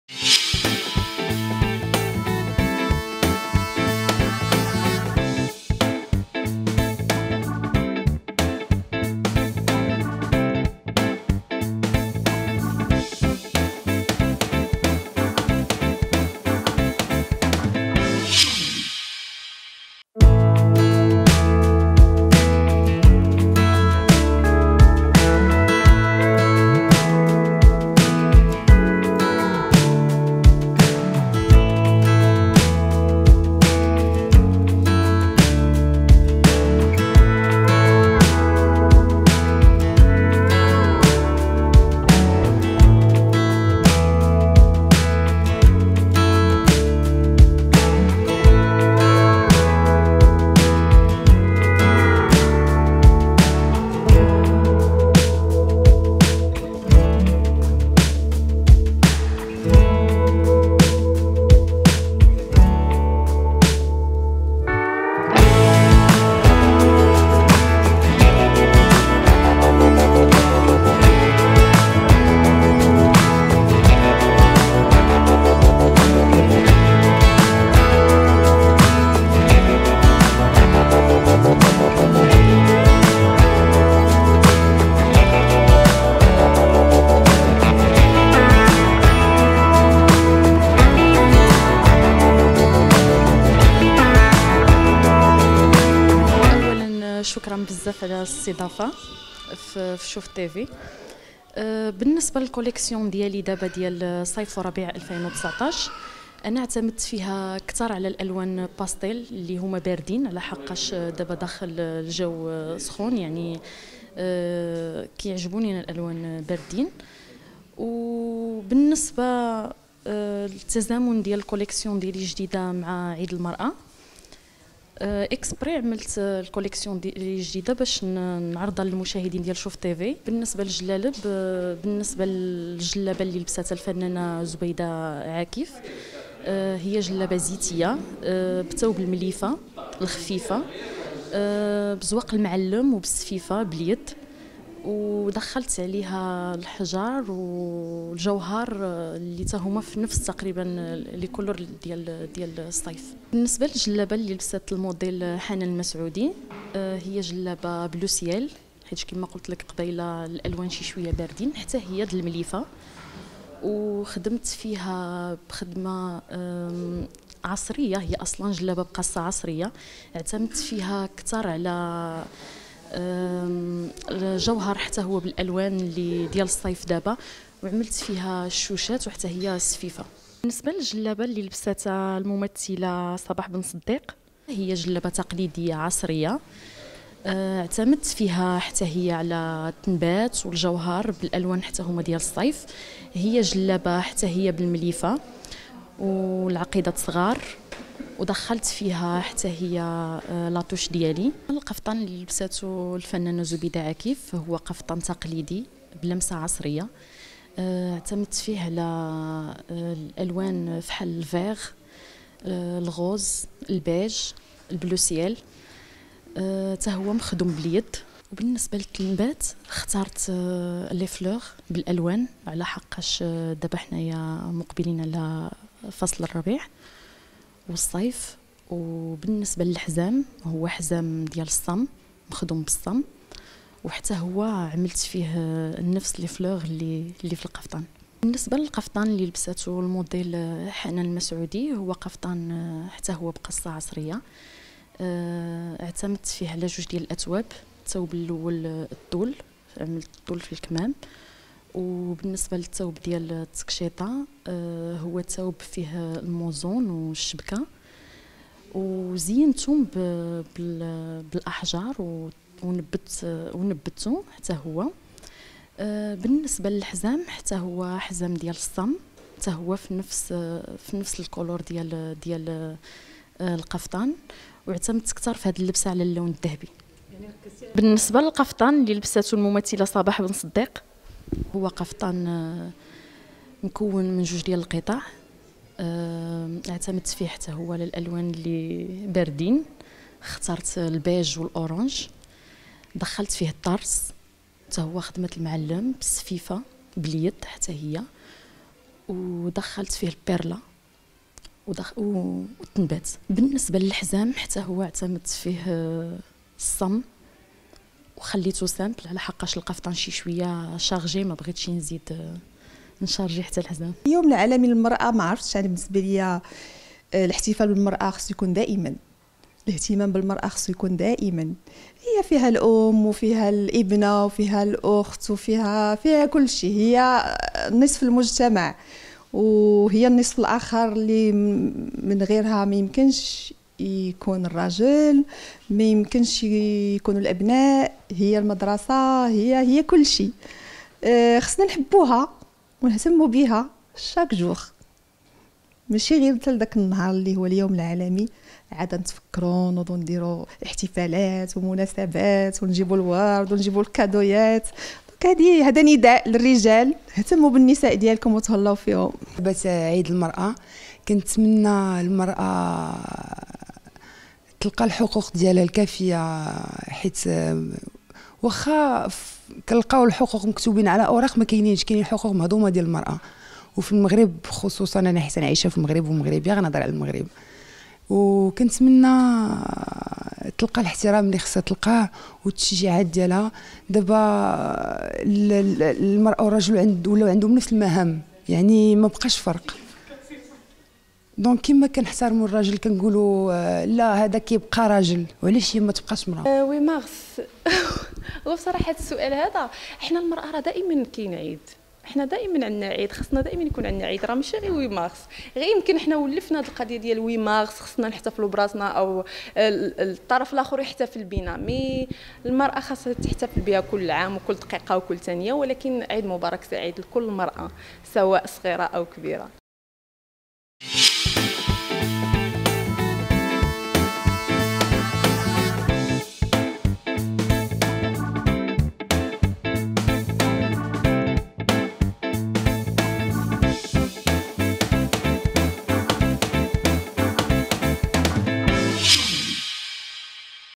فنانات مغربيات يرتدين تشكيلة جديدة من القفاطين
اختارت مصممة شابة  بمناسبة الاحتفال بالمرأة موعدا لعرض تشكيلتها الجديدة من القفطان المغربي؛ وعن هذه المناسبة عبرت فنانات مغربيات عن ضرورة الاهتمام واحترام المرأة المغربية كما دعت أن يكون الاحتفاء بالمرأة يوميا وليس مرة في السنة